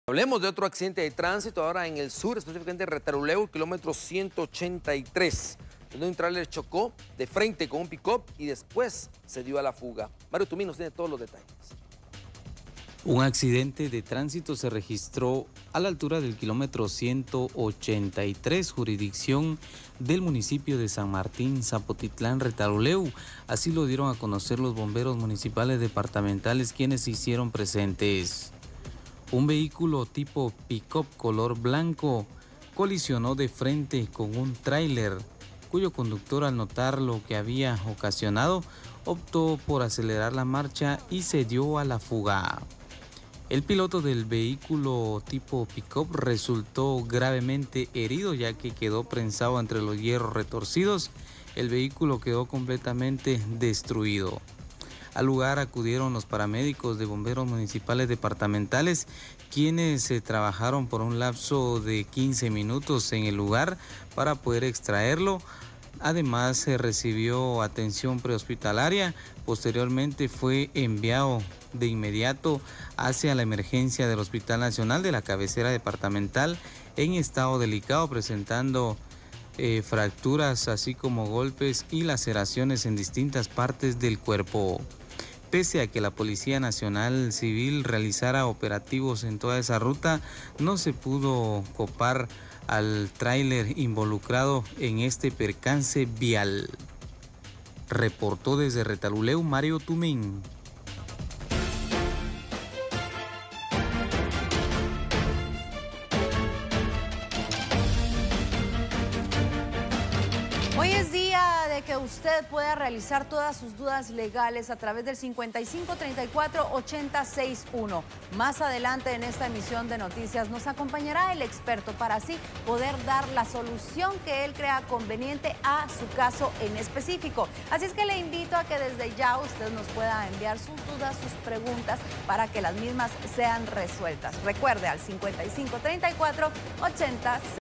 Tráiler chocó de frente con un picop en Retalhuleu
El piloto del picop resultó grave ya que quedó prensado en la cabina del Vehículo

El aparatoso accidente  se registró en el kilometro 183 jurisdicción del Municipio de San Martin Zapotitlán, departamento de Retalhuleu.

Bomberos Municipales remitieron al herido al hospital nacional en estado critico. Visita nuestro sitio